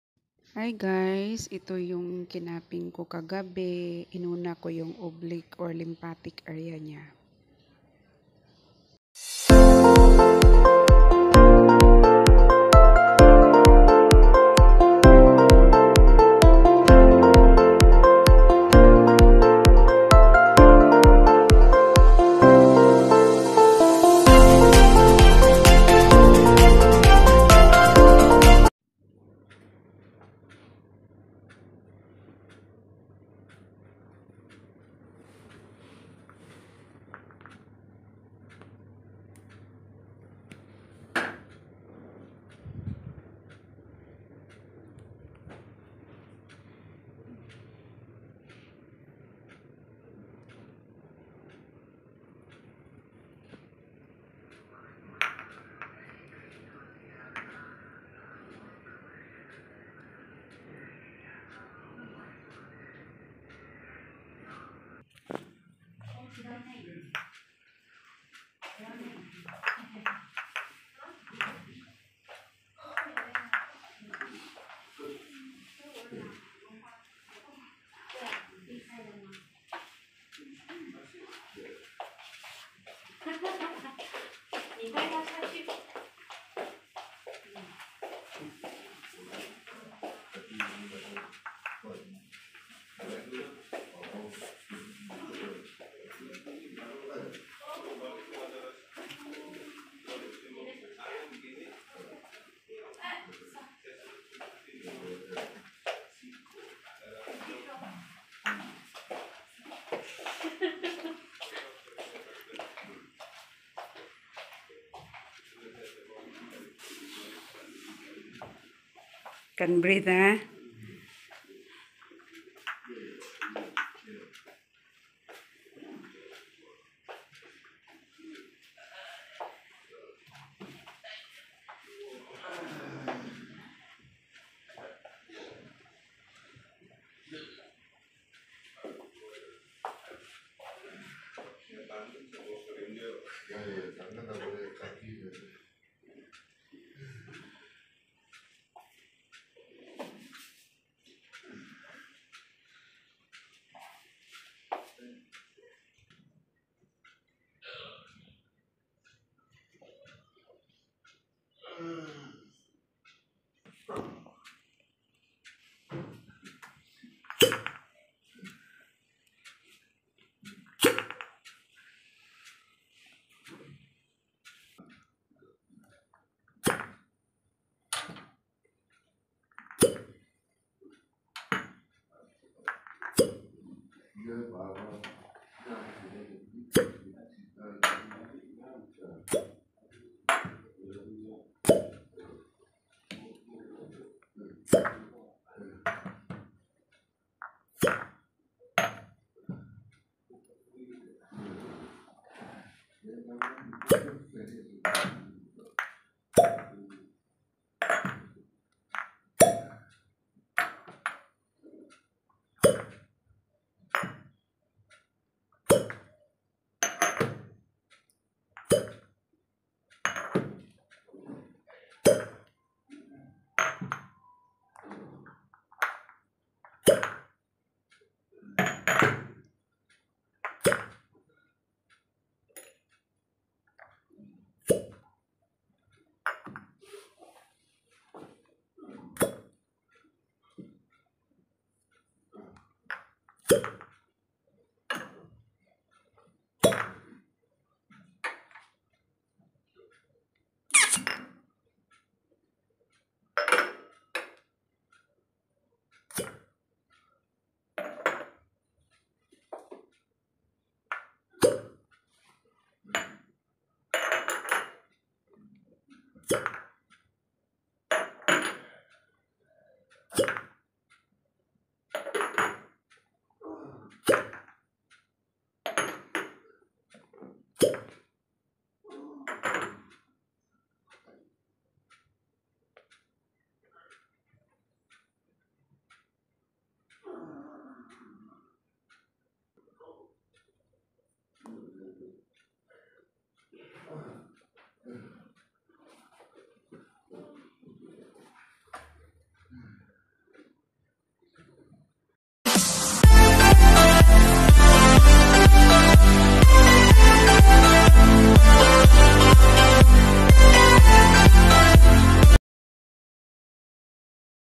THREE HOURS MASSAGE WITH CUPPING
My regular client was too tired and stressed,so I helped him at ease all his sufferings.Massage plus cupping treatment is one of the solutions from tiredness.